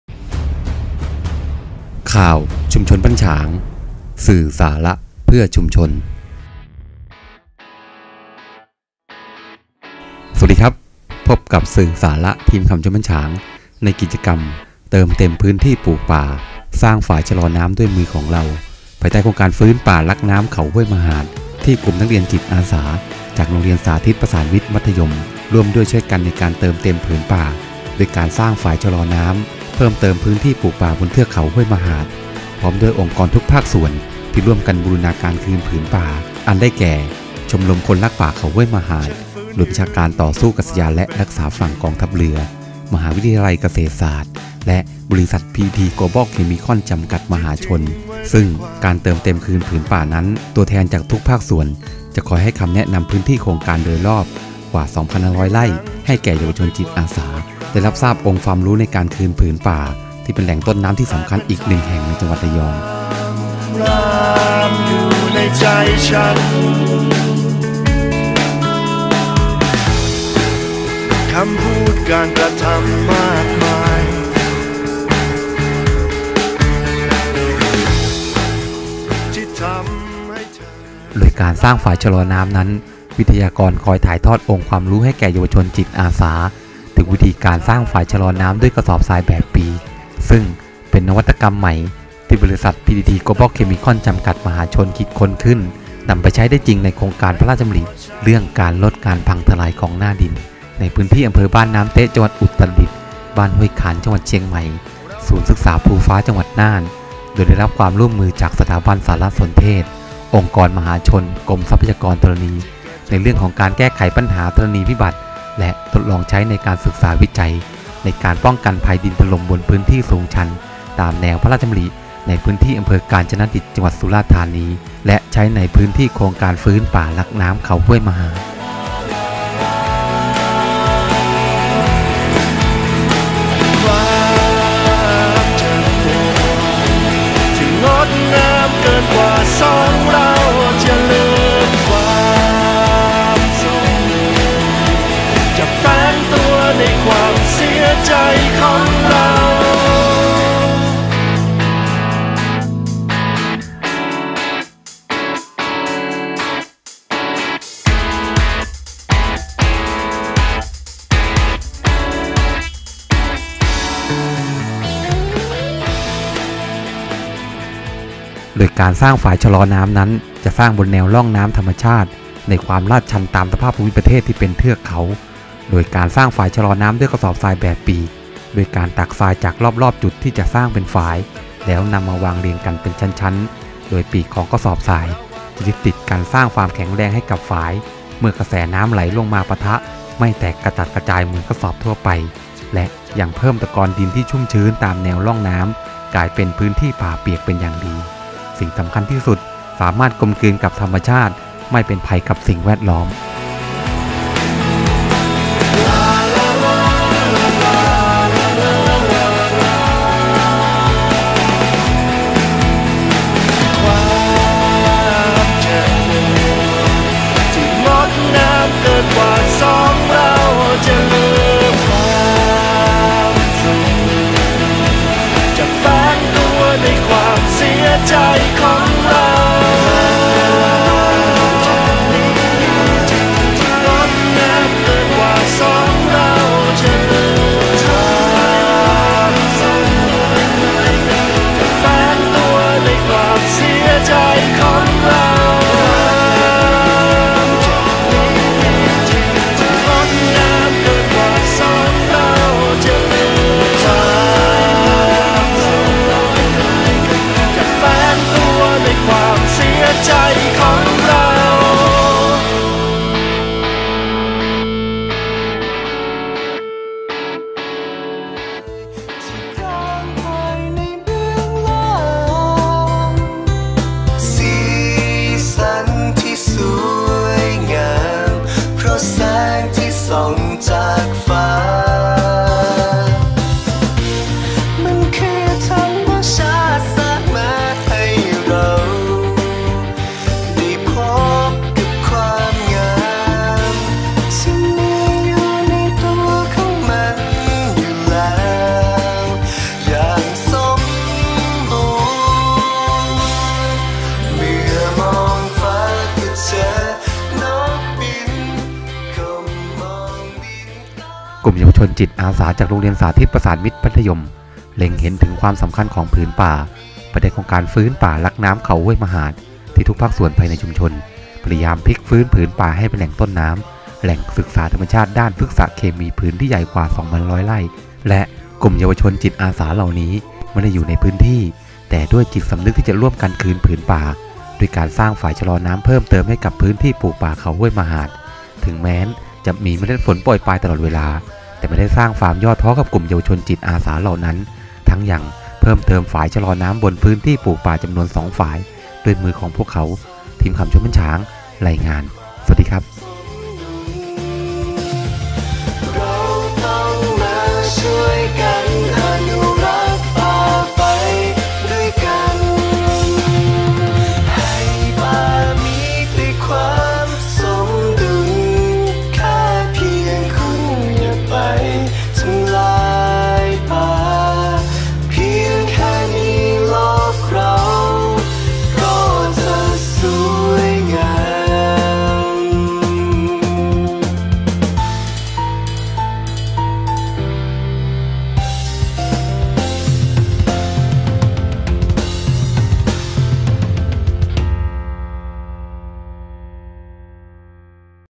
เติมเต็มผืนป่าสร้างฝายชะลอน้ำ
เมื่อกลุ่มเยาวชจิตอาสา  โรงเรียนสาธิตมหาวิทยาลัยศรีนครินทรวิโรฒ ประสานมิตร (ฝ่ายมัธยม) เดินทางร่วมกับ ทุกภาคส่วนที่ทำข้อตกลงบูรณาการคืนผืนป่าภายใต้โครงการฟื้นป่ารักน้ำเขาห้วยมะหาด อันได้แก่ชมรมคนรักษาป่าเขาห้วยมะหาด หน่วยบัญชาการต่อสู่อากาศยาน และรักษาฝั่งกองทัพเรือ มหาวิทยาลัยเกษตรศาสตร์ และ บริาัทพีทีที โกลบอลเคมิคอนจำกัดมหาชน ร่วมสร้างฝายชละน้ำเพิ่มเติมให้ผืนป่าเขาห้วยมะหาด